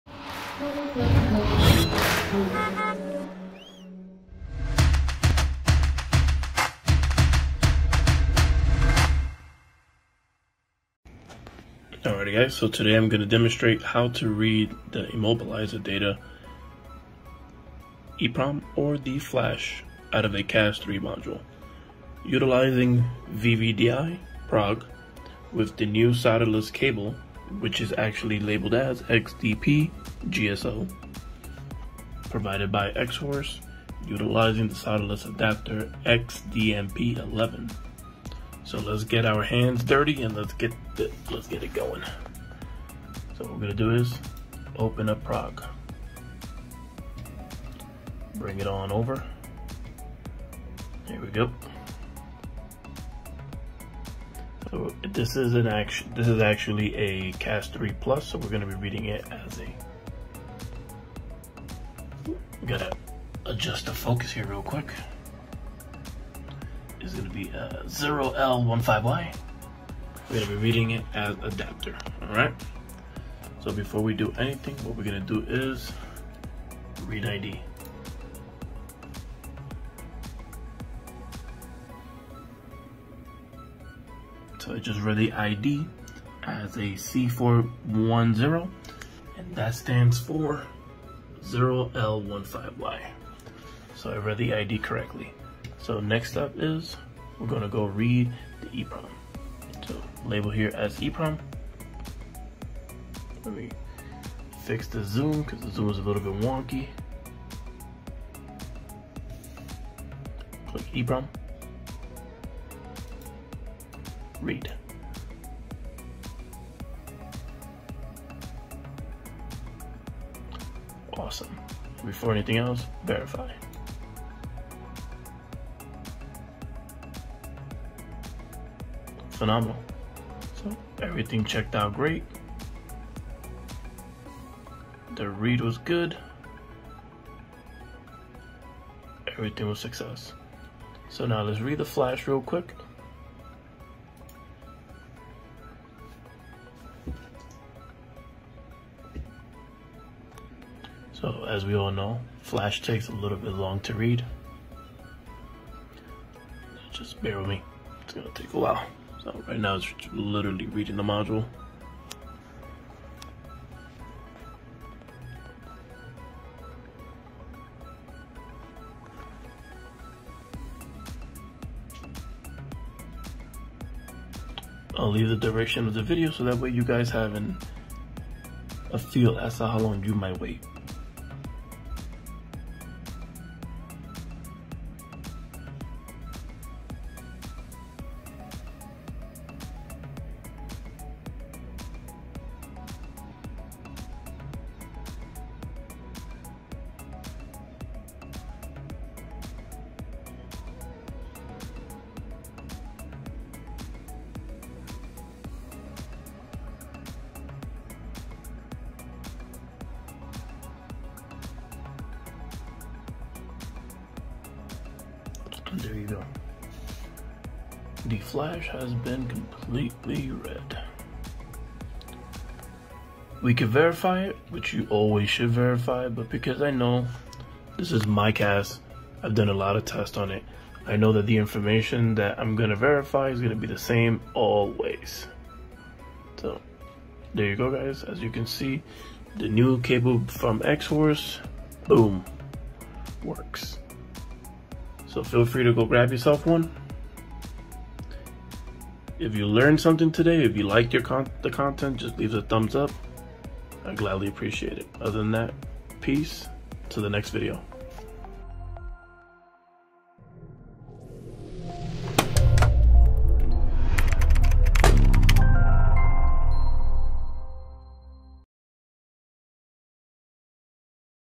How To Read BMW CAS3 System w Xhorse VVDI PROG Solderless Adapter Cable EEPROM - Xhorse Fan Sub 3
This is an Xhorse VW Solder-Free Full Adapters for the VVDI Key Tool Plus and the VVDI Mini PROG

These adapters support access and retrieval of BCM2 IMMO data without the original key ID as well as the exchange of the BCM2 function in future updates.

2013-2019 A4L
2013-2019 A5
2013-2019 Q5
(BCM2 version: 0711, 0650, 0641) support add key and all keys lost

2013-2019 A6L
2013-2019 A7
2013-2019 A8
(BCM2 version: 0825, 0824, 0827) support get BCM2 IMMO data when all keys lost